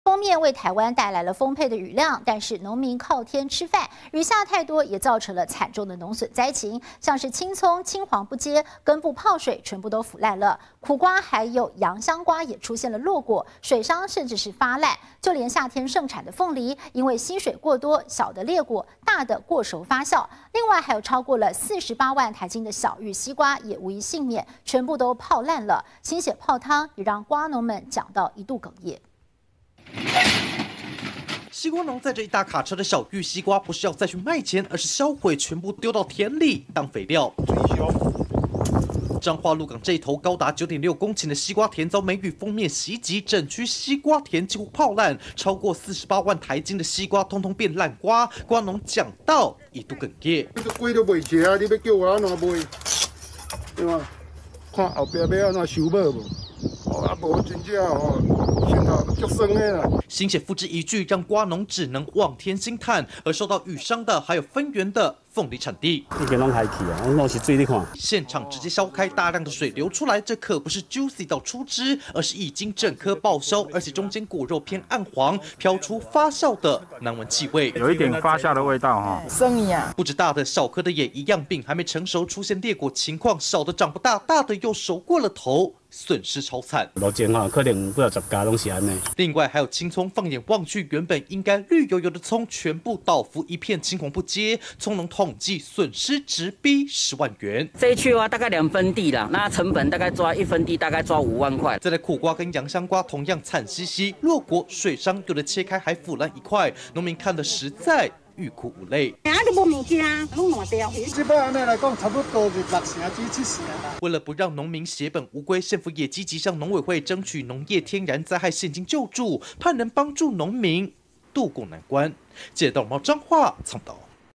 上波梅雨降雨彈! 鹿港逾48萬斤西瓜變爛瓜
上一波梅雨鋒面為台灣帶來豐沛雨量，但農民靠天吃飯，雨下太多，也造成慘重農損，像是青蔥青黃不接，根部泡水全部腐爛，苦瓜和洋香瓜也出現落果、水傷甚至發爛，就連夏天盛產的鳳梨，因為吸水過多，小的裂果，大的過熟發酵，另外還有超過48萬台斤的小玉西瓜也無一倖免，全部變成爛瓜，心血泡湯也讓瓜農講到心酸，一度哽咽！